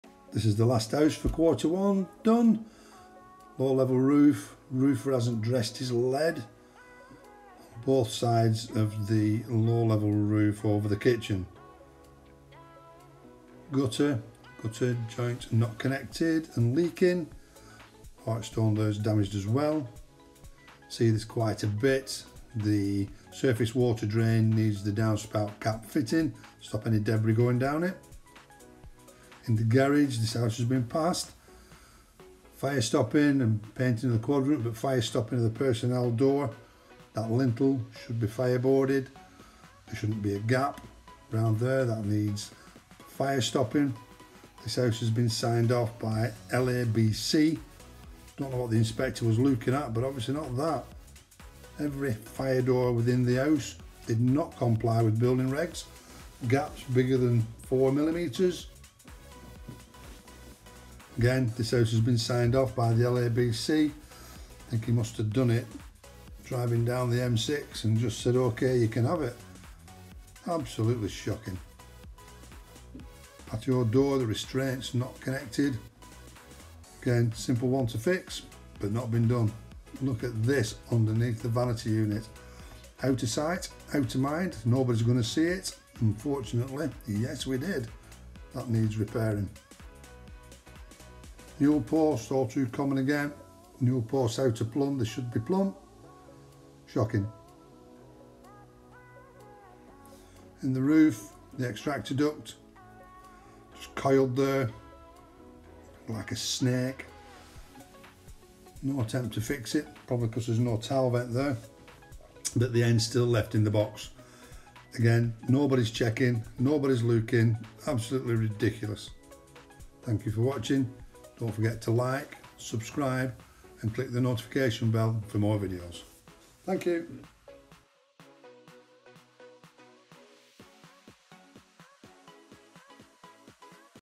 The Last House for Quarter 1 2022 SHOCKING!
This is the last house to be snagged in Q1, another house that has been signed off by Local Authority Building Control (LABC) probably from a 747 flying overhead considering the number of breaches in Building Regulations.

Our Services:
Description